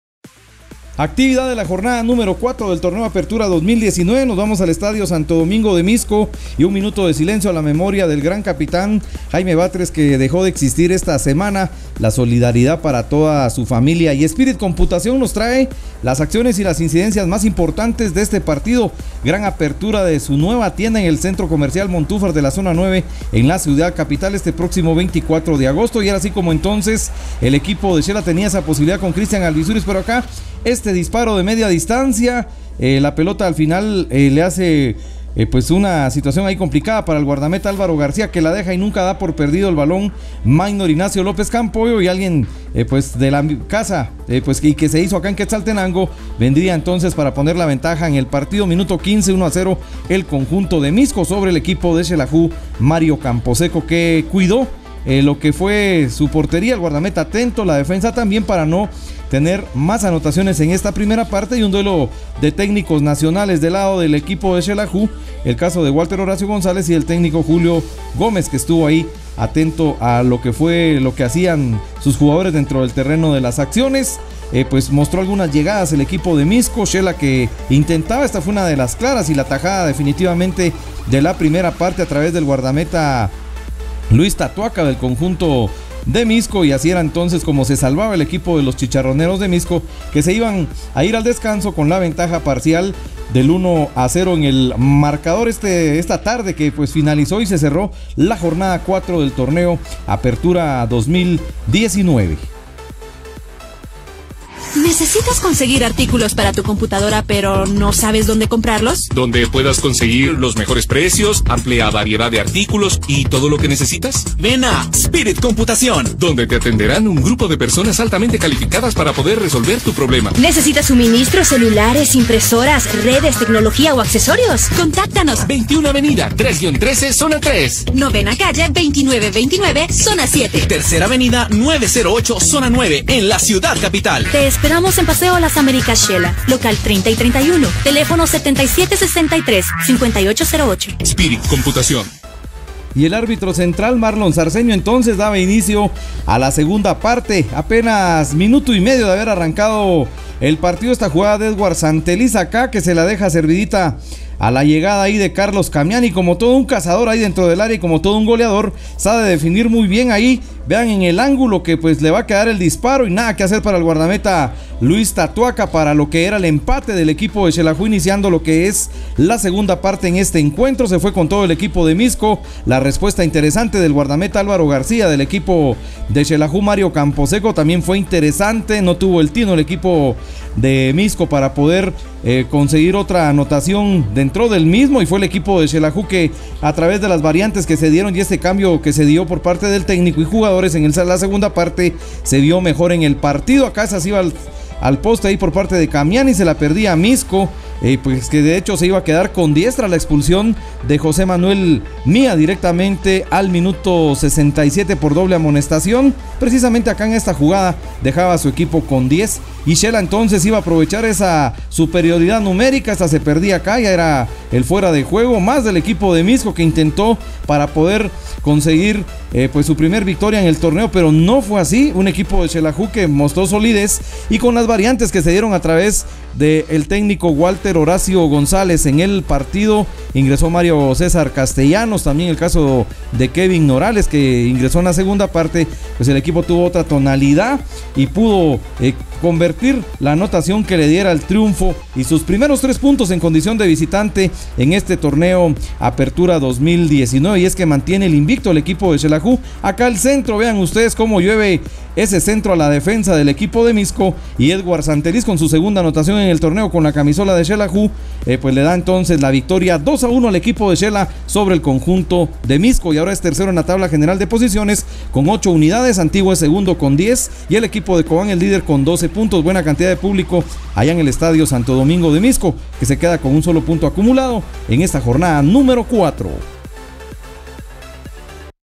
Mixco FC 1 - Xelaju MC 2 Jornada 4 Apertura 2019
Spirit Computación te presenta
Resumen Jornada 4   Apertura 2019
Mixco FC 1 - Xelaju MC 2